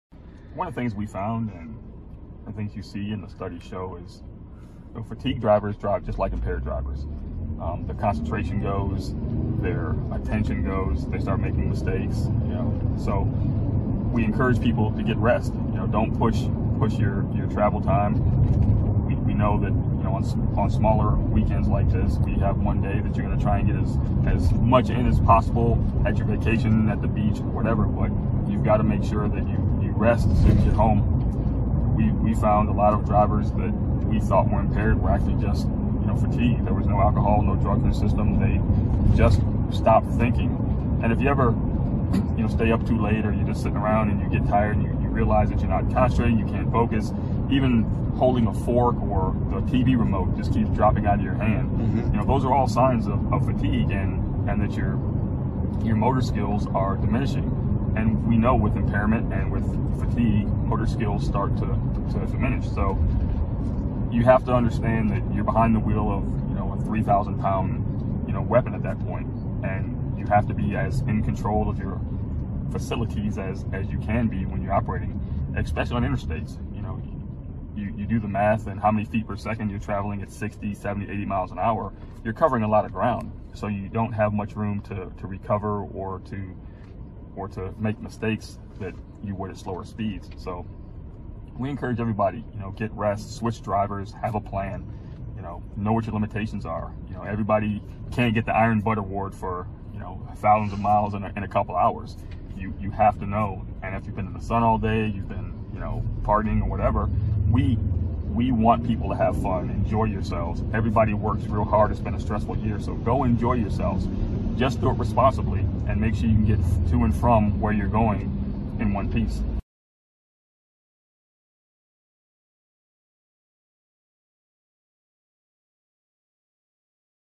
Captain Lee Peters Driving 3
Law enforcement is out in force this holiday weekend. Stafford Sheriff's Office Captain Lee Peters talks to us about driving while super tired.